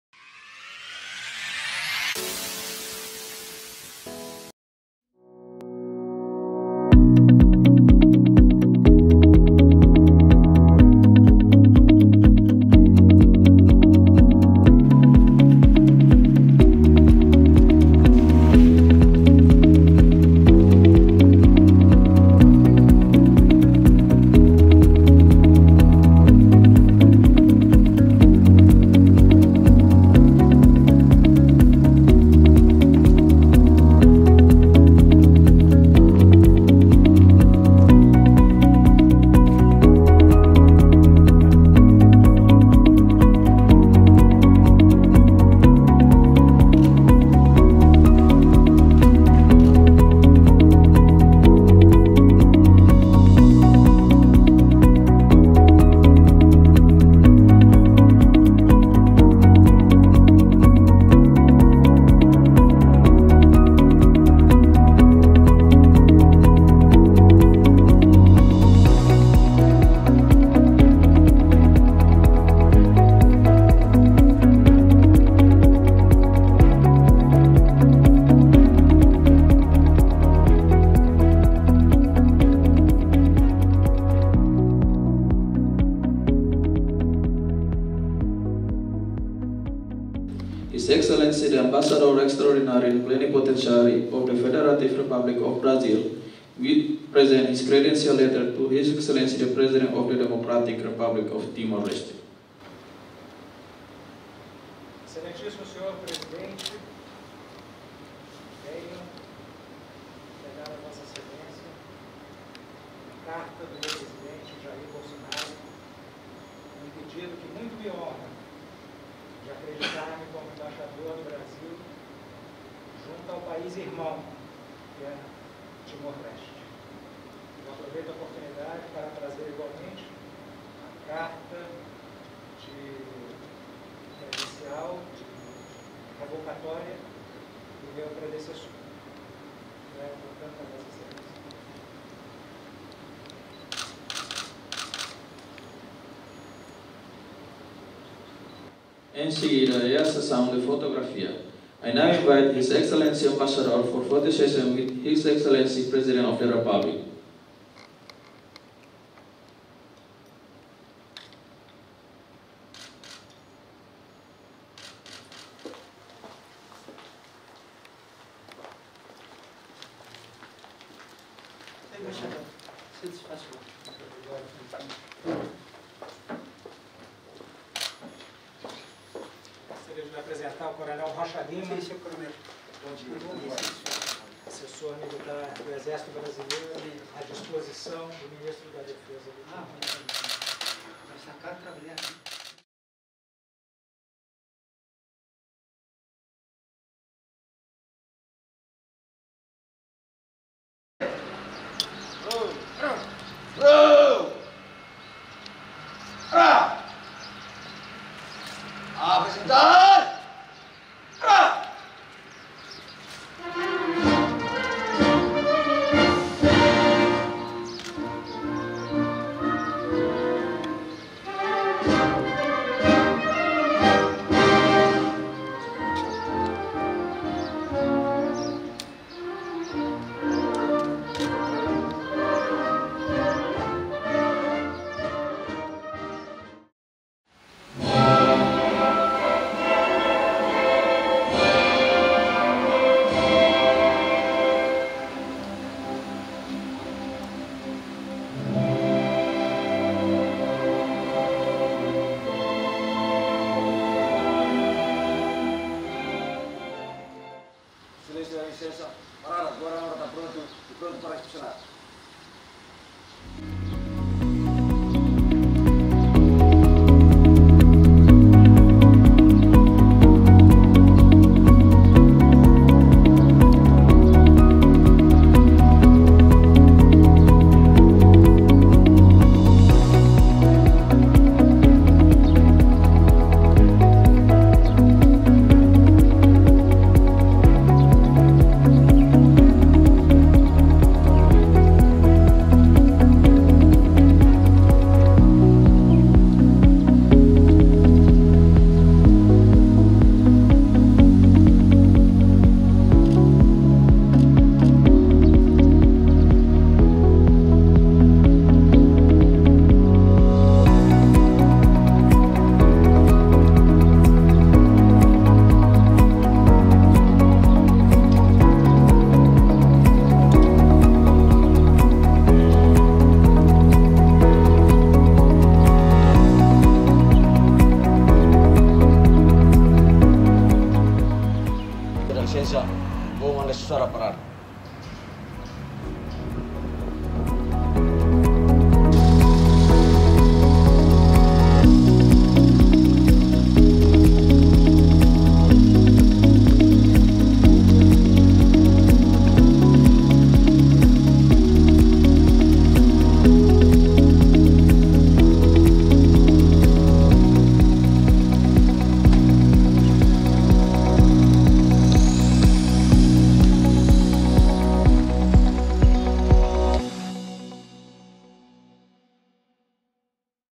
PR Lú Olo Simu Karta Kredensiál Husi Embaixadór Brazil
Relasaun diplomátikas entre Timor-Leste Brazil estabelese iha 20 maiu 2002 ho loke ninia embaixada iha Díli iha tinan hanesan. Timor-Leste harii nia embaixada iha Brazil iha tinan 2008 nia rohan.
Governu Brazil, iha 2000, liuhusi Ajénsia Brazileira ba Kooperasaun identifika área prioridade ba koperasaun téknika iha edukasaun, agrikultura, formasaun profisionál no administrasaun públika.
Projetu dahuluk ne’ebé hala’o mak implementasaun Sentru Promosaun Sosiál, Formasaun Profissional no Dezenvolvimentu Emprezarial iha Becora ho parseria Servisu Nasionál ba Aprendizajen Industiral (SENAI).
Governu Brazil nia asisténsia bilateral ba Timor-Leste mak doasaun komputadór Tribunal Rekursu, livru ba Institutu Estudu Diplomátikus, bolsas estudus ba timoroan, apoiu téknika ba Parlamentu Nasionál, Defensoria Públika, Ministériu Defeza no Universidade Nasional Timor-Lorosa’e.
Kooperasaun bilateral foun sei konsentra iha setór agrikultura oinsá bele hamenus dependénsia ba hahan importasaun ho baze familiar, serbisu ho Comissão Anti Corrupção, programa maestrado iha àrea edukasaun iha Universidade Nasional Timor-Leste no portugés iha funsaun públika.
Embaixadór Charles Kevin Blackstone hahú nia karreira diplomátika iha 1991 iha Servisu Relasaun Esterior Departamentu Estadu ba Estadus Unidus Amerika. Molok mai Timor-Leste sai responsavel ba kargu oioin  no nia postu ikusliu mak Diretór Ezekutivu ba Bureau do Sudeste Asiático e Pacífico iha Departamentu Estadu.
Embaixadór Mauricio Medeiros de Assis inisia nia karreita diplomátika iha 1996. Husi kargu oioin ne’ebé asume, Mauricio sai responsavel Xefe Divizaun Informatika, Ministru Segunda Klase tanba meresimentu no Konsuladu-Jeral iha Sydney, Australia entre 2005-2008 no Xangai, Repúblika Populár Xina, entre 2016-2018.